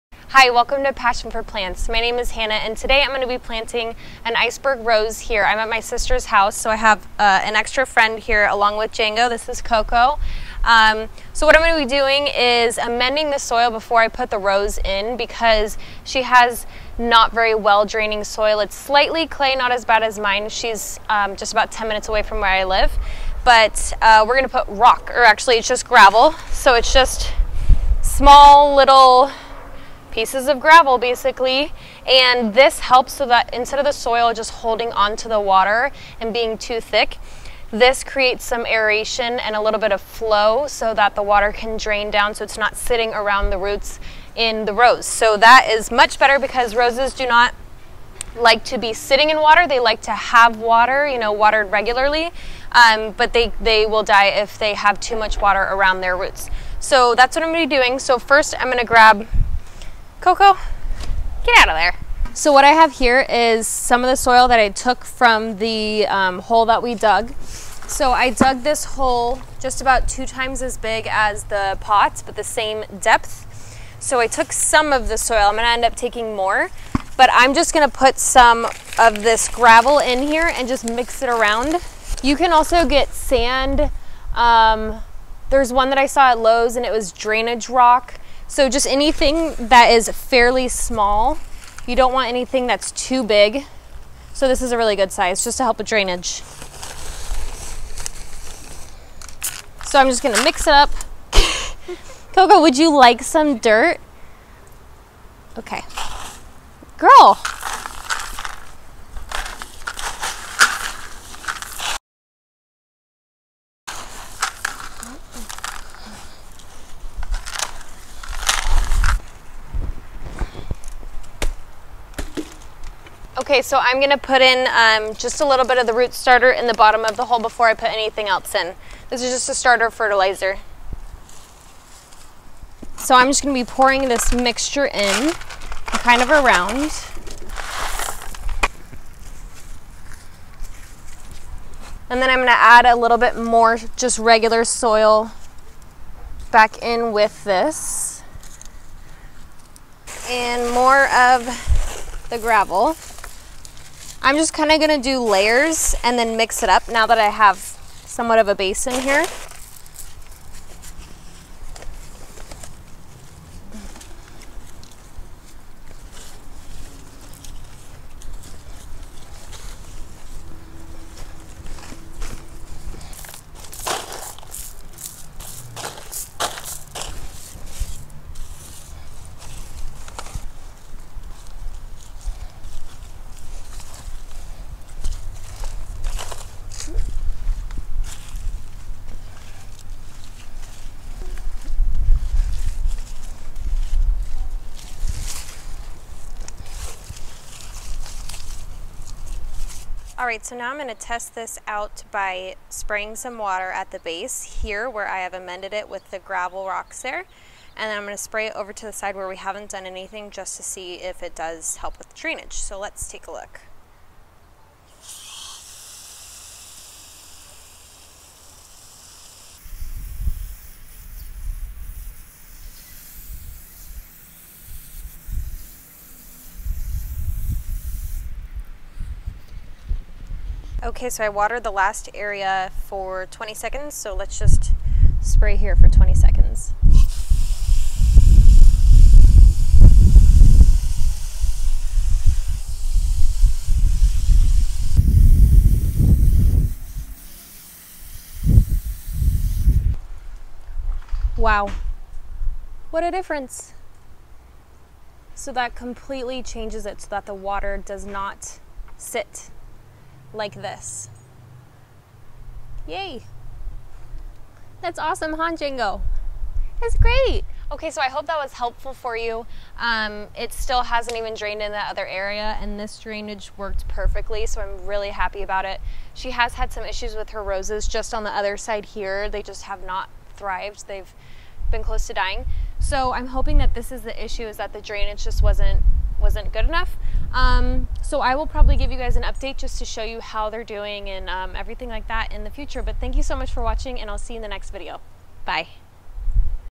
How To AMEND SOIL When Planting Roses
Dr Earth Root Starter Fertilizer
Drainage Rock (Similar)

Here is how I amend my soil in order to have better drainage for roses!